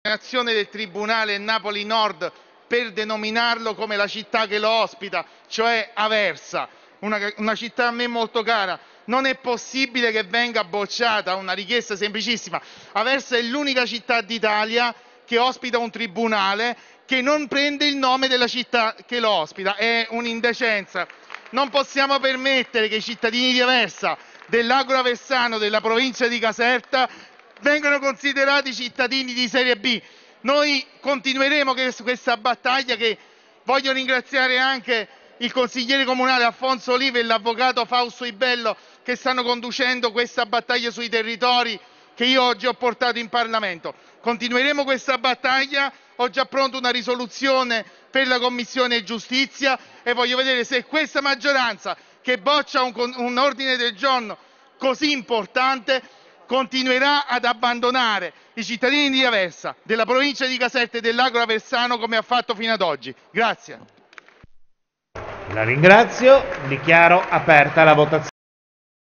Aversa (CE) - Tribunale, bocciata alla Camera la richiesta di cambio denominazione (25.11.21)
- Aversa (Caserta) - Bocciato alla Camera dei Deputati l’ordine del giorno riguardante il cambio dei denominazione del “Tribunale di Napoli Nord” a “Tribunale di Aversa”. “Purtroppo, il Pd, partito di riferimento del sindaco di Aversa ha bocciato l’ordine del giorno in palese contrasto con quanto affermato dallo stesso in Consiglio comunale”, ha commentato Alfonso Oliva, consigliere comunale di Fratelli d’Italia, la cui mozione (redatta insieme all’avvocato Fausto Ibello, responsabile Dipartimento Giustizia e Rapporti col Tribunale di Fdi) era stata votata all’unanimità dal Consiglio comunale di Aversa. Continua su (25.11.21)